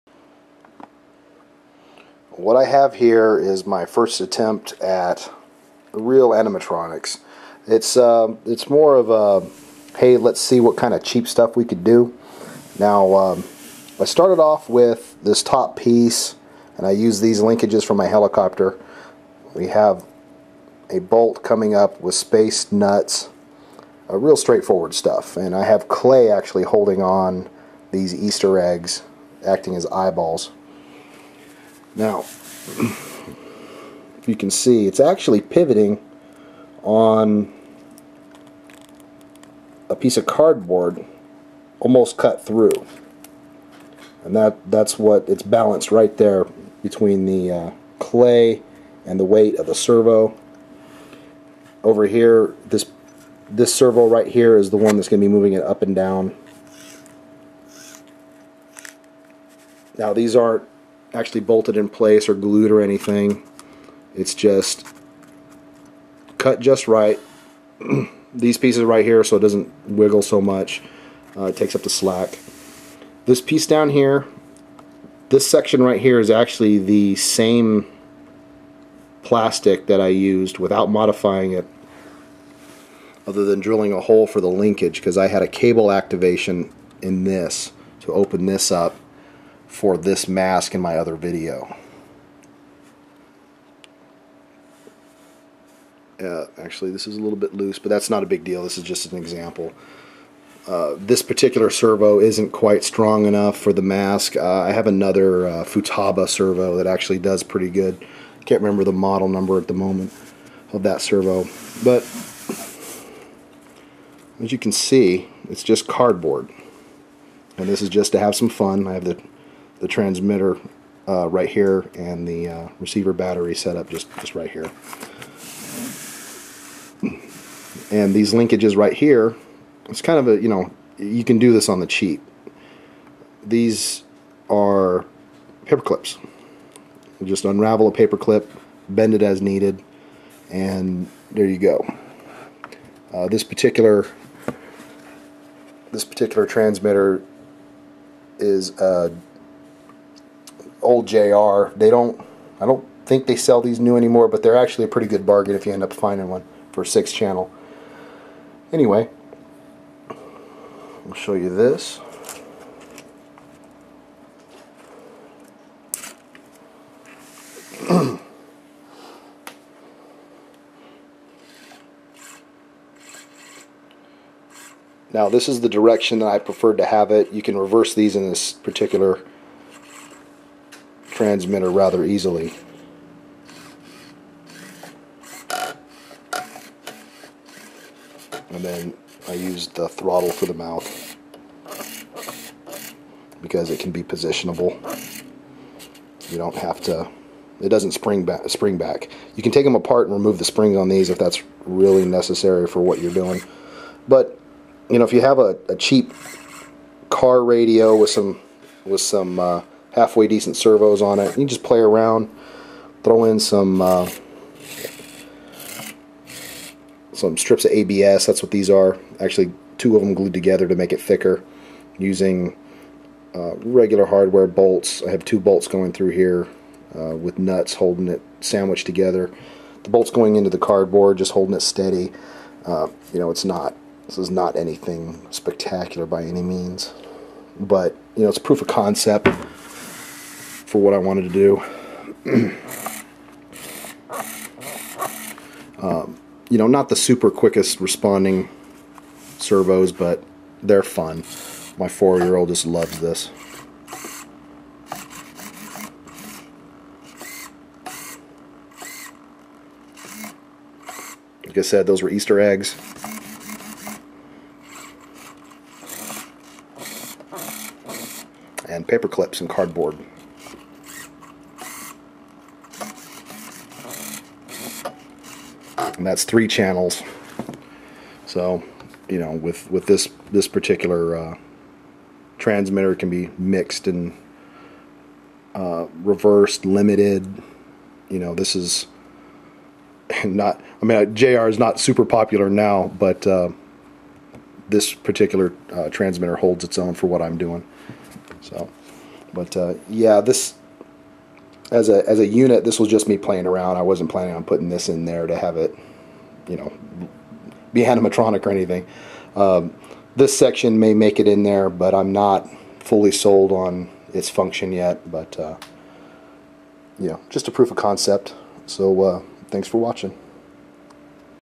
Cardboard animatronic test setup
I wanted to make a cheap/disposable setup that would be fun and allow me to explore the linkage while I wait for my parts order.

If you're interested in learning more about animatronics, I recommend checking these guys out: